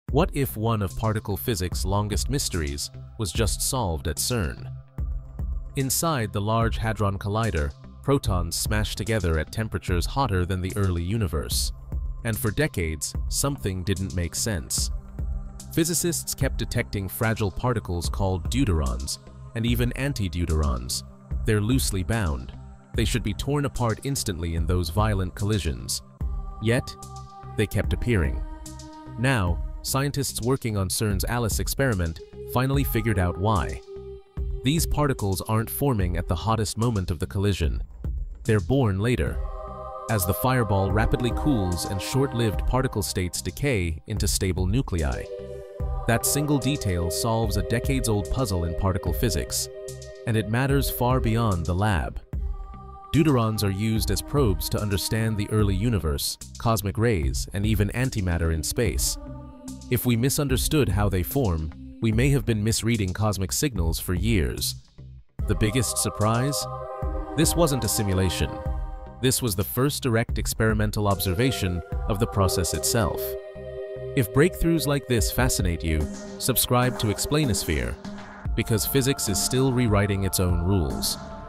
CERN Just Solved a 50 Year Old Physics Mystery
For decades, physicists were confused by fragile particles appearing inside the most violent collisions ever created.
Now, scientists at CERN’s ALICE experiment have directly observed how they form — solving a mystery that’s puzzled particle physics for over 50 years.

This discovery could change how we interpret signals from the early universe, cosmic rays, and antimatter in space.